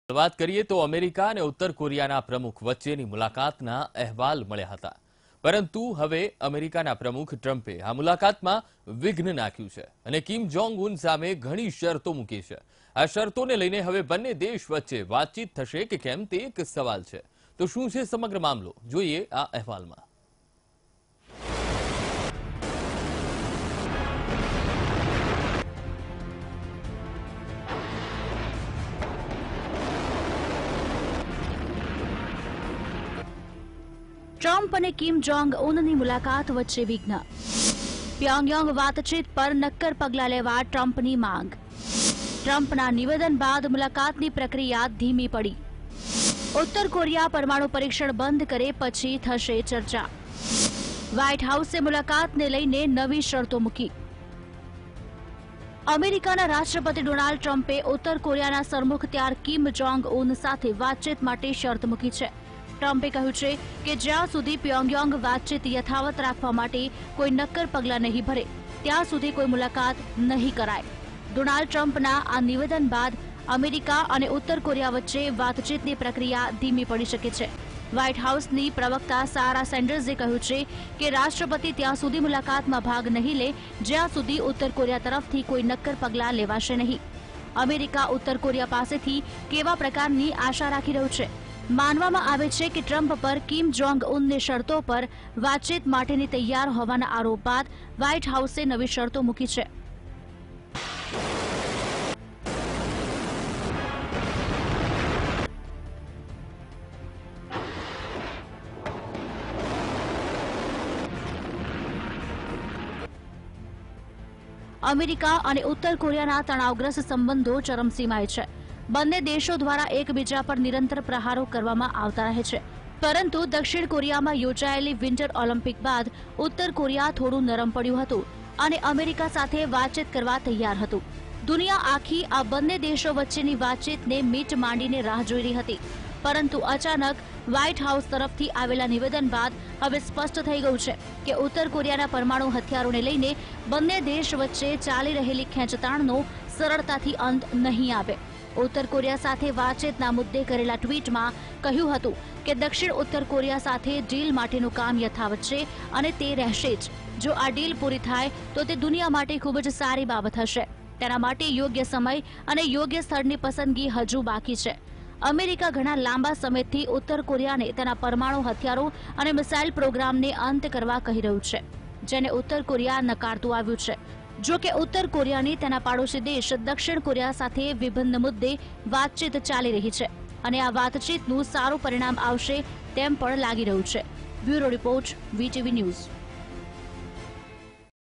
Donald Trump: won`t talk with north korea untill north korea doesn`t stop nuclear program | Vtv News
Donald Trump: won`t talk with north korea untill north korea doesn`t stop nuclear program